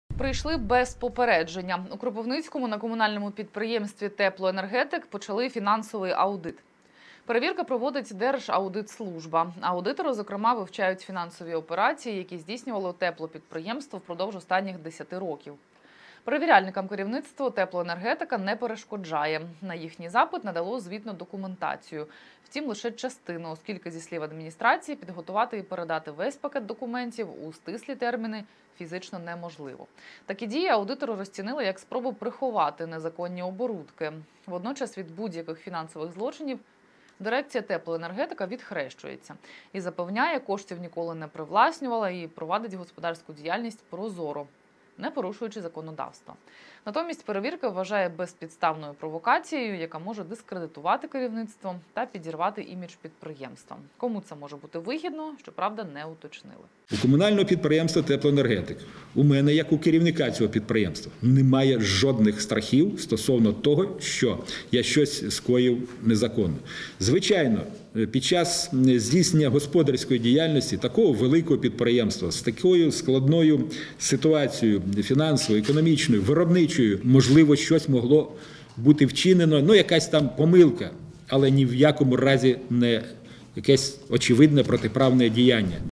У Кропивницькому Держаудитслужба підозрює КП «Теплоенергетик» у привласненні бюджетних коштів
Нині на підприємстві триває фінансовий аудит. Аудитори, зокрема, вивчають фінансові операції, які здійснювало теплопідприємство впродовж останніх 10 років. Перевіряльникам керівництво «Теплоенергетика» не перешкоджає. На їхній запит  надало звітну документацію. Втім, лише частину, оскільки, зі слів адміністрації, підготувати і передати весь пакет документів у стислі терміни фізично неможливо. Такі дії аудитори розцінили, як спробу приховати незаконні оборутки. Водночас від будь-яких фінансових злочинів дирекція «Теплоенергетика» відхрещується. І запевняє, коштів ніколи не привласнювала і провадить господарську діяльність прозоро, не порушуючи законодавства. Натомість перевірки вважає безпідставною провокацією, яка може дискредитувати керівництво та підірвати імідж підприємства. Кому це може бути вигідно, щоправда, не уточнили.